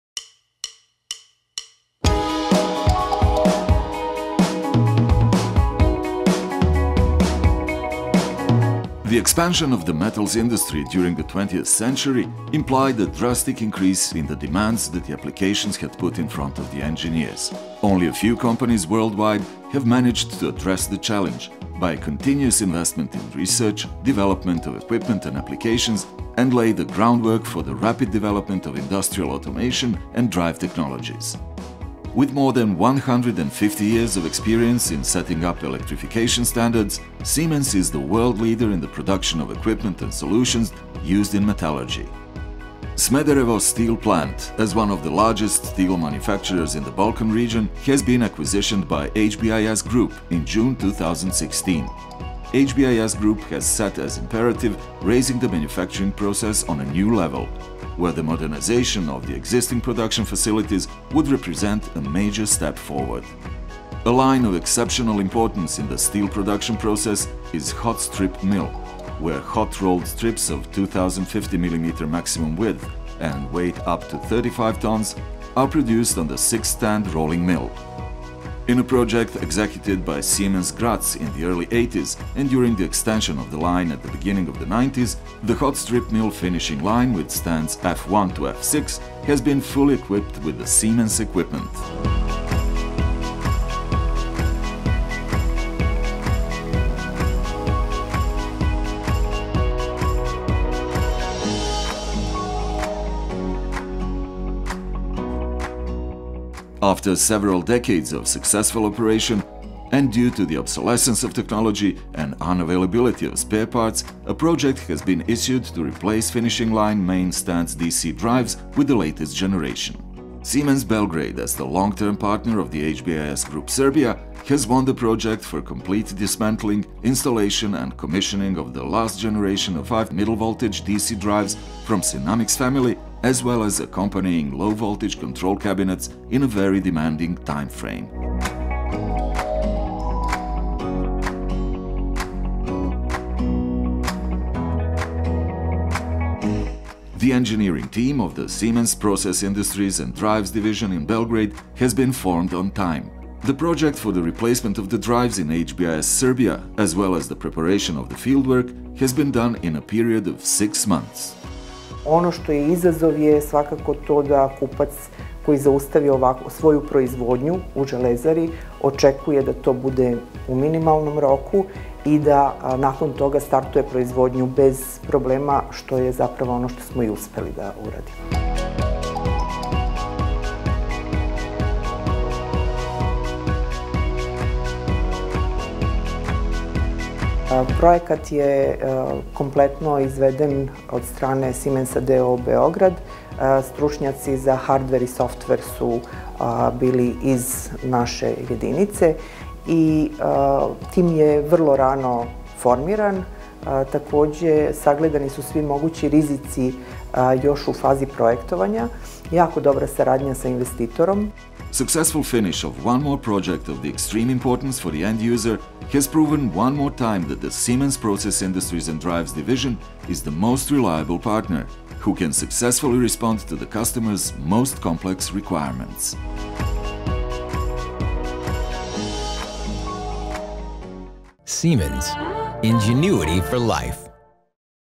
Siemens Serbia, Modernization of Hot Strip Mill finishing line - HBIS
Siemens Serbia as the long-term partner of HBIS Group Serbia, has won the project for complete dismantling, installation and commissioning of the last generation of five middle voltage DC drives from SINAMICS family, as well as accompanying low-voltage control cabinets, and in a very demanding time frame. Successful finish of one more project of the extreme importance for the end user, has proven one more time that Siemens Serbia is the most reliable partner, who can successfully respond to the customers most complex requirements.